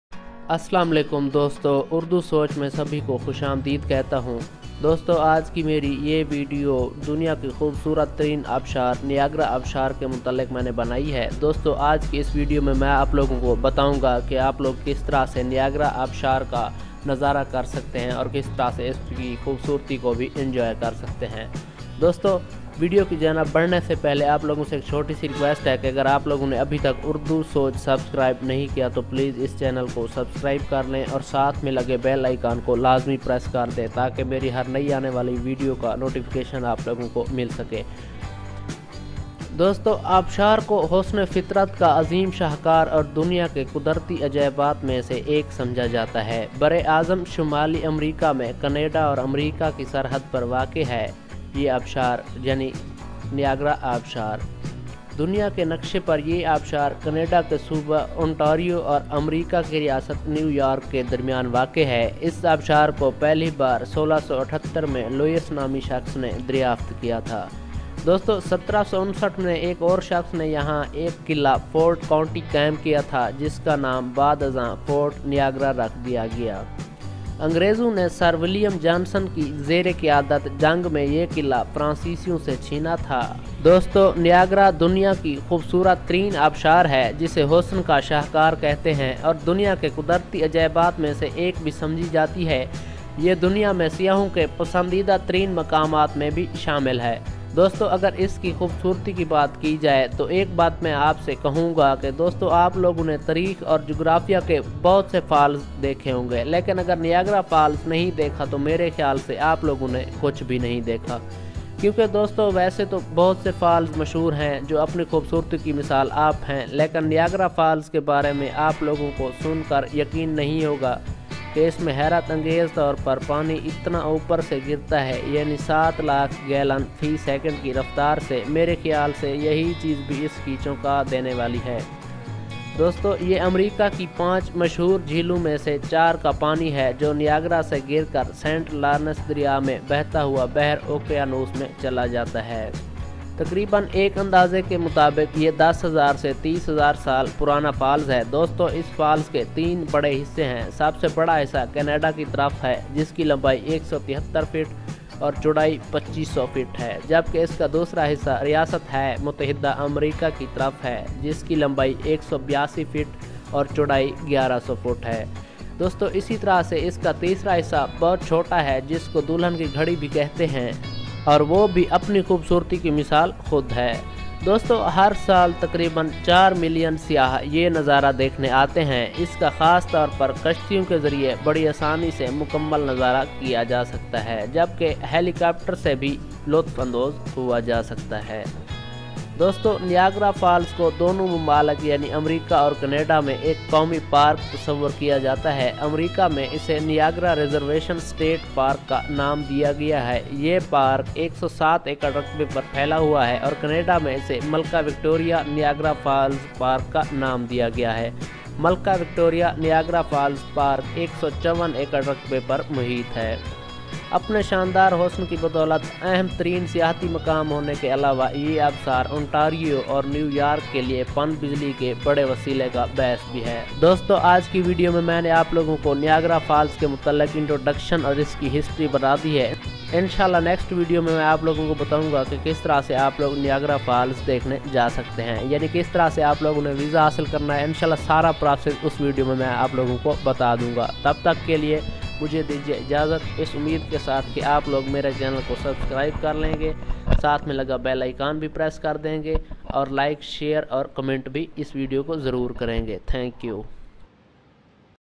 Niagara Falls Documentary in Urdu | World of Waterfalls - Layyah studio
Niagara Falls water Fall in Urdu Which Side is Better in Canada
Niagara Falls ,Great place to relax, bring out-of-town relatives, and do some outlet shopping